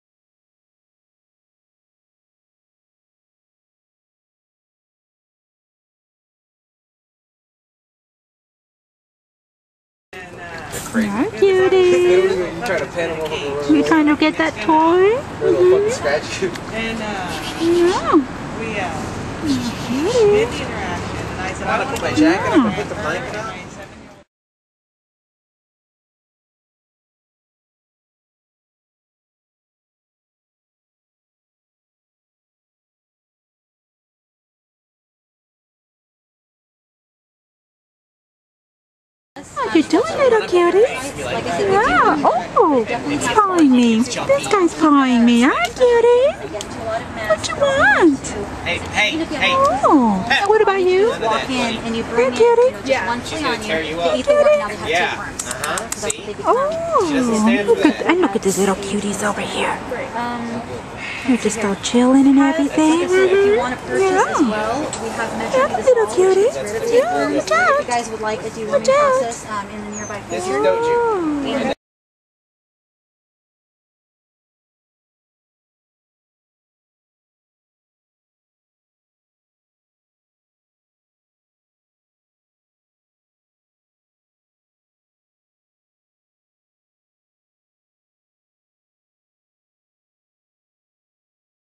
Dog & Cat Adoption Events - Biweekly
Huge thanks goes out to Pet Me, Please! who continuously offer their parking lot for these ongoing events.
Dogs, cats, puppies and kittens offered for adoption each have a unique story.  Some have been relinquished by previous owners facing personal challenges, while others were one step away from being euthanized.  Not spaying or neutering pets are often times the major reason these animals end up in these situations.
It is here, at these events, where prospective and permanent homes can become a reality for many of these 'displaced' but innocent animals.